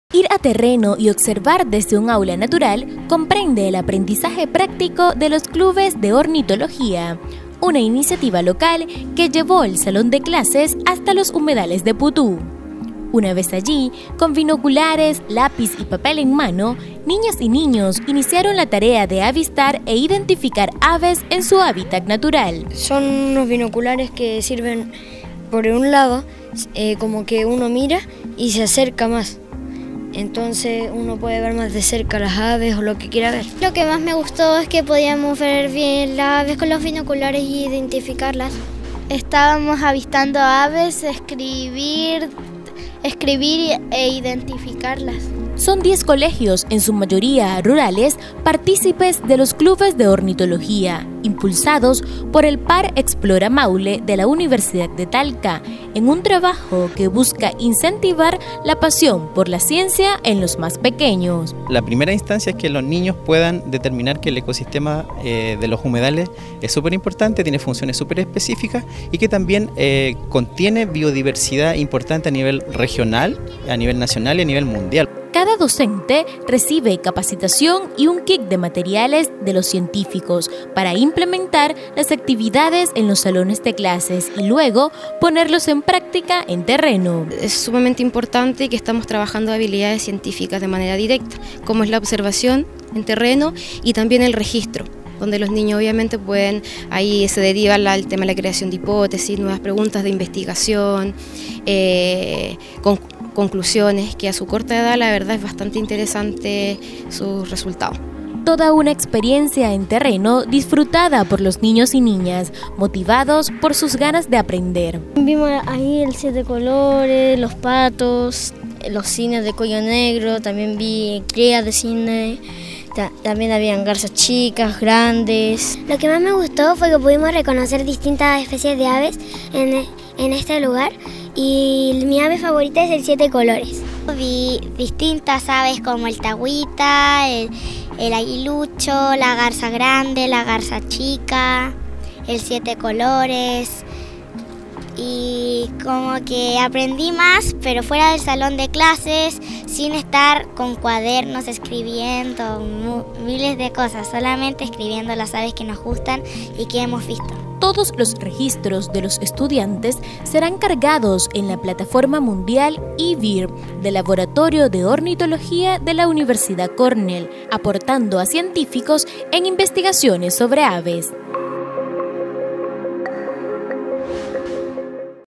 Niños y niñas hacen avistamiento de aves en Humedales de Putú.
Ir a terreno, observar y registrar aves comprende el aprendizaje práctico de los clubes de ornitología del Par Explora Maule. Uno de los clubes es “Bioavecillas del Maule” del Colegio Constitución quienes en sus prácticas llevaron el salón de clases hasta los Humedales de Putú. ¿Qué te parece esta iniciativa?